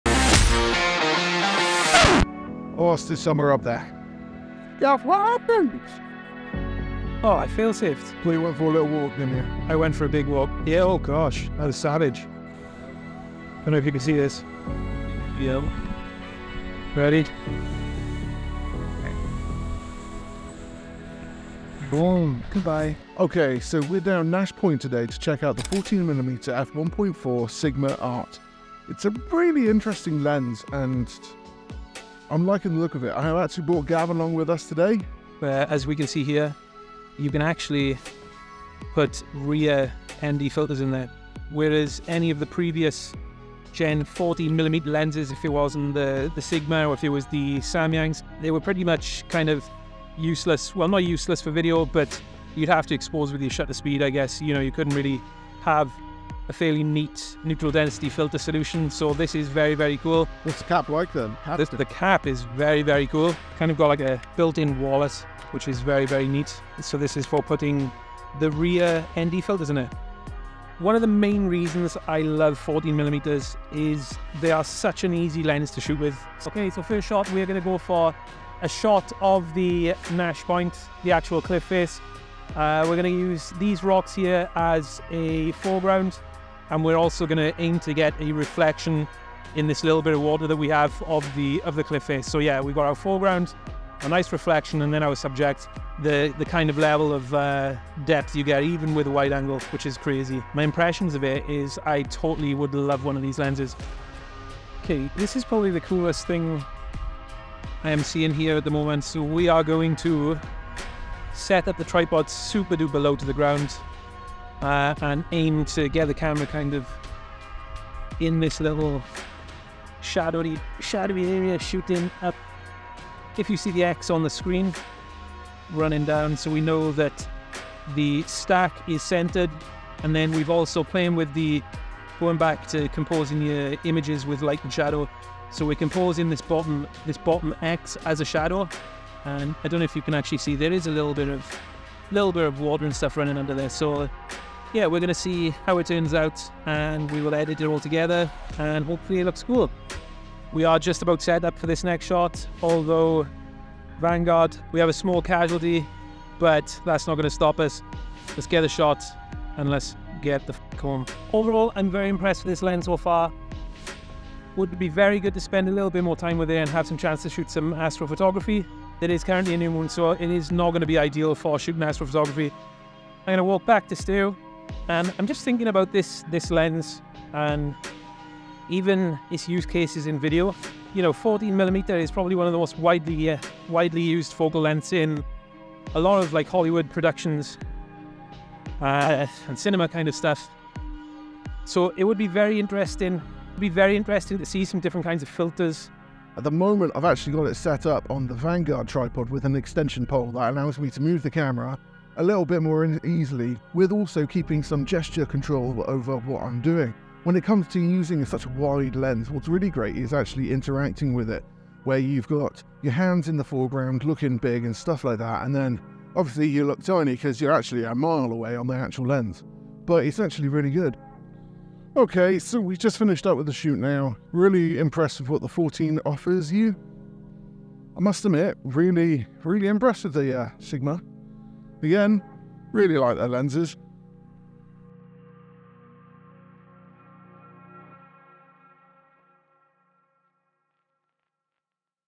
Sigma 14mm f1.4 - THE BEST LANDSCAPE OPTION?
Here master landscape photographer Gavin Lee tests out the new Sigma Art 14mm f1.4 DG DN lens. This lens stands as a testament to @sigmauk's state-of-the-art design. The size and quality of the glass is completely outstanding.

Within the the video Gavin gives you a few useful tips as to his process of composition, imparting a few words of wisdom.

Timecode
00:00 Intro
00:24 Sigma ART 14mm f1.4 product overview
01:15 Composing landscape photography
02:05 Using leading lines in photography
03:00 Gav's thoughts
03:15 The Firby Shot - 360 rotating platform